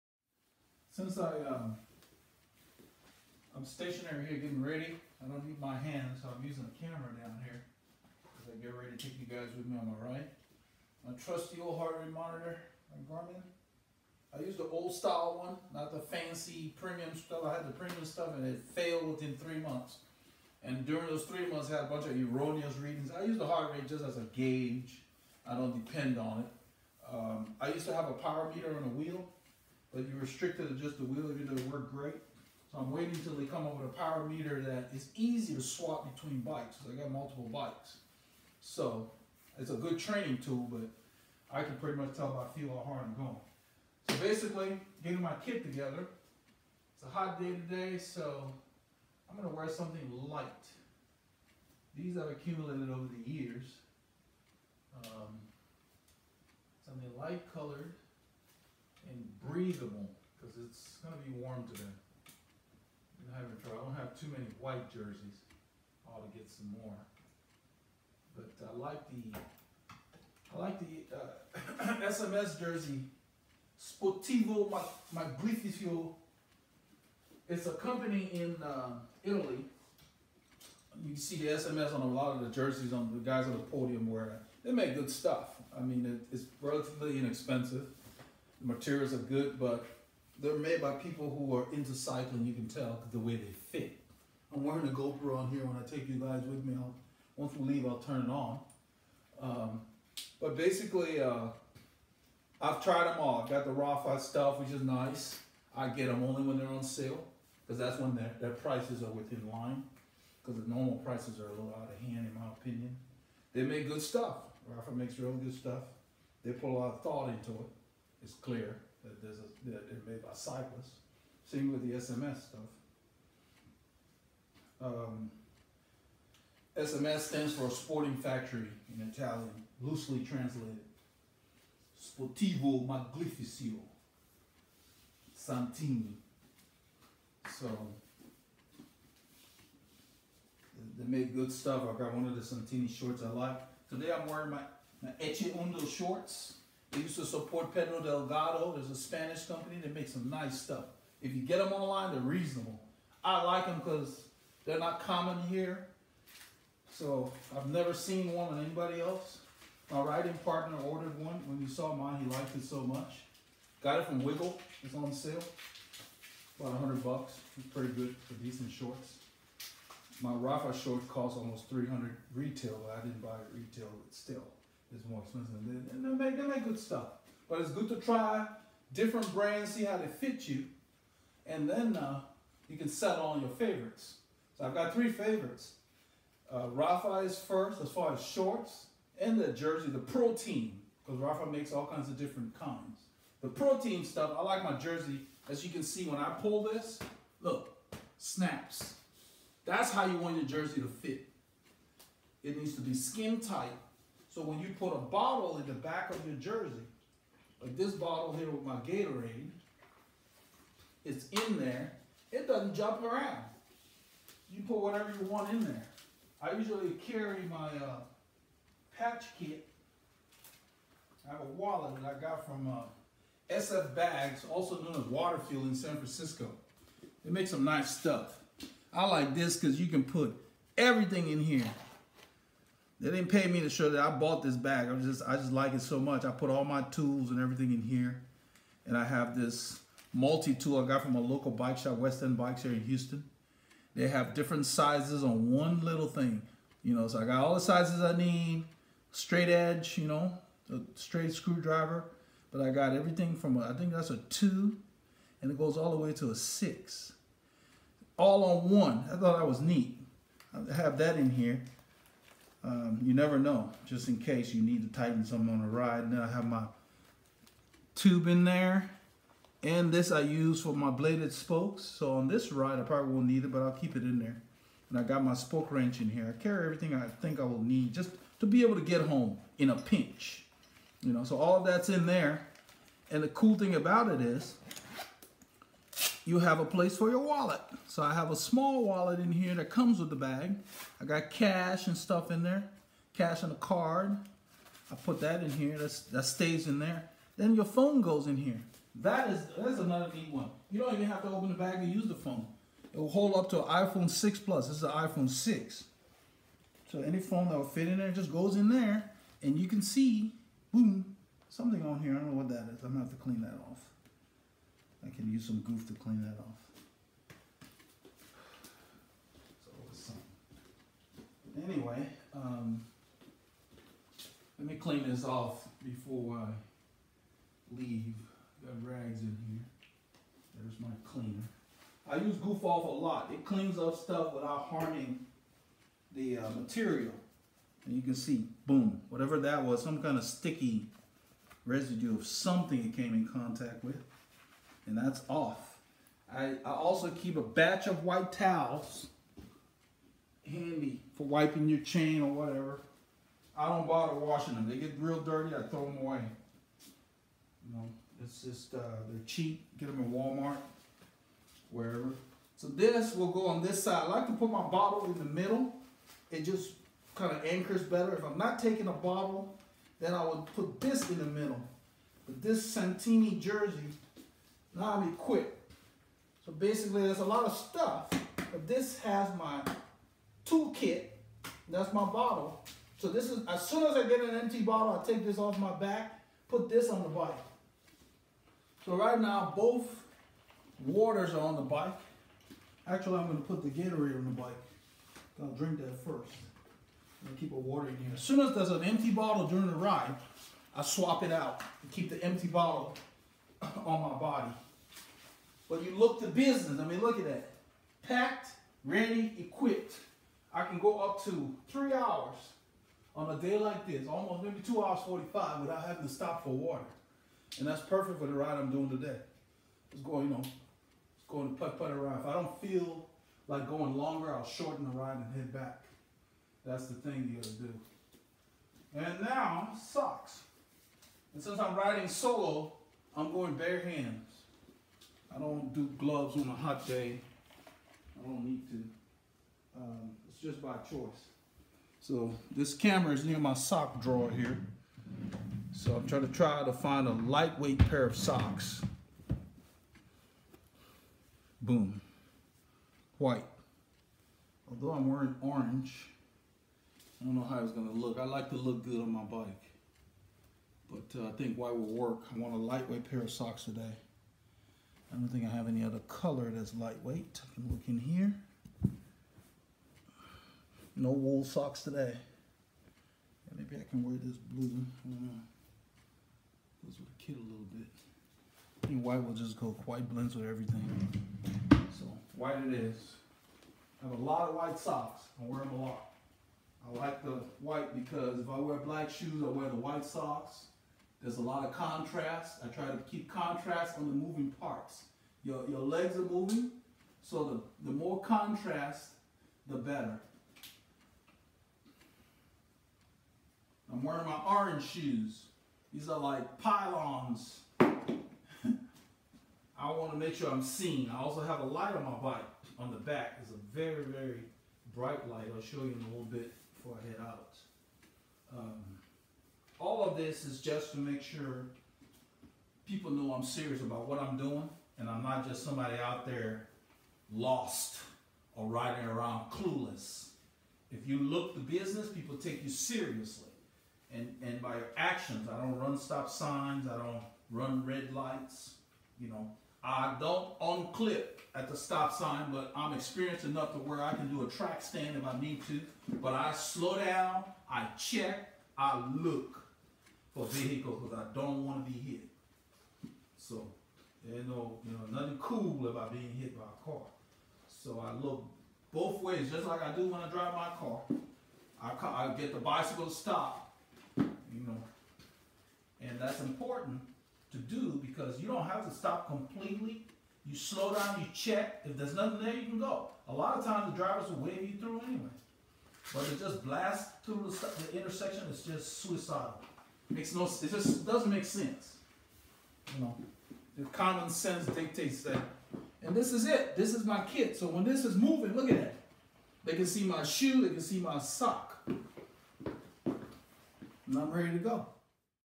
Summer cycling clothing - What I wear & carry when cycling in Summer
I walkthrough on how to prepare for a ride on a hot day.

Main Release Schedule:
Monday 6am CST
Friday 6am CST